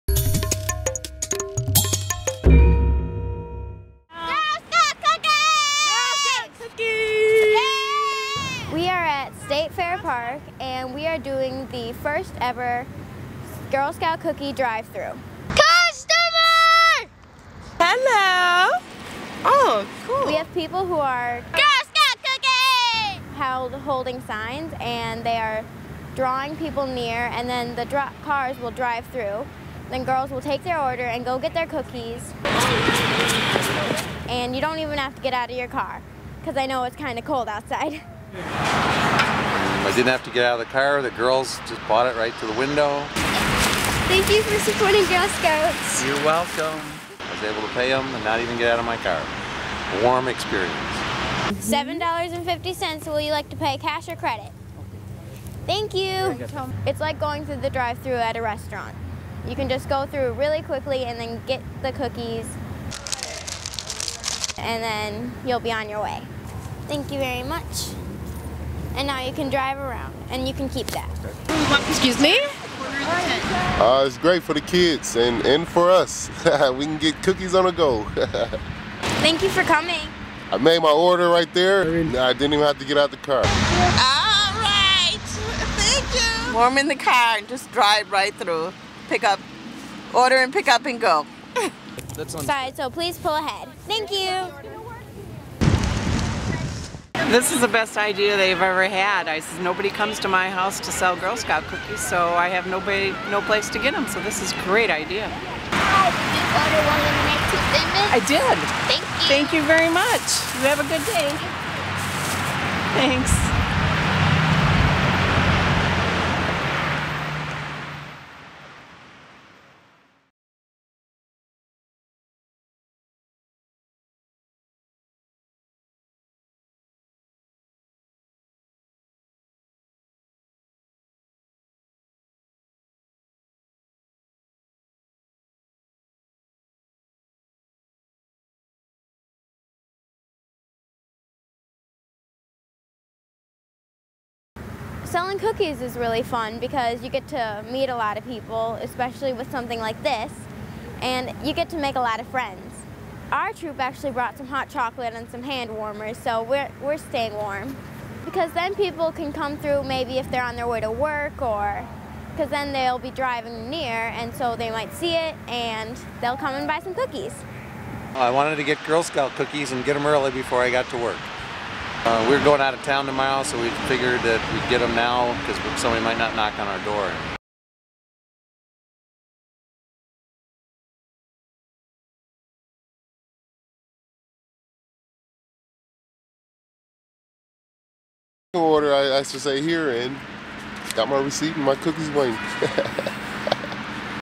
Drive-through Girl Scout cookies
The Girl Scouts of Wisconsin Southeast launched its first drive-through cookie booth at Wisconsin State Fair Park in West Allis on Thursday. Video by Mike De Sisti